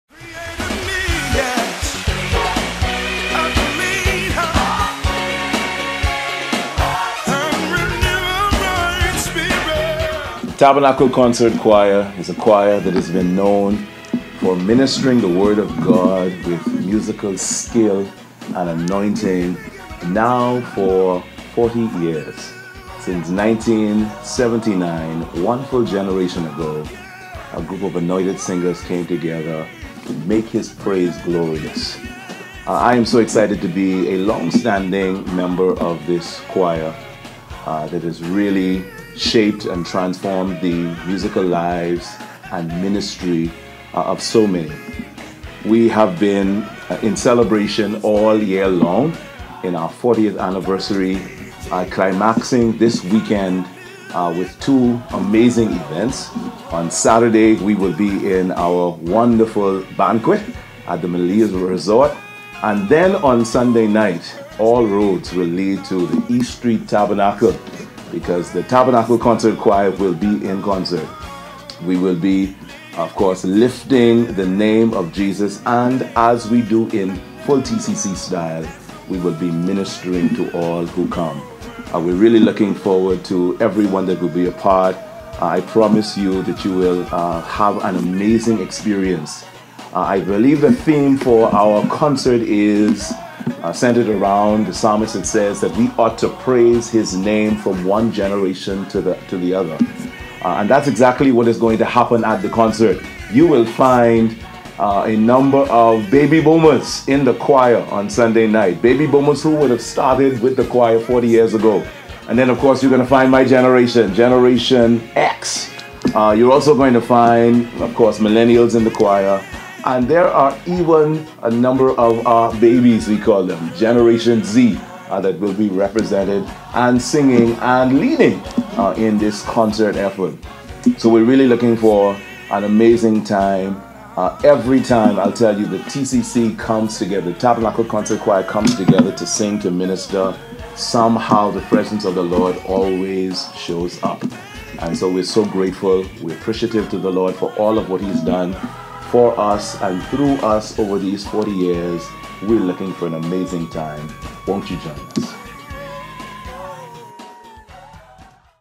TCC 40th Anniversary Promo 3 - Ian Ferguson
Former Assistant Director, Pastor Ian Ferguson, invites all to attend TCC's 40th Anniversary Concert, November 3, 2019.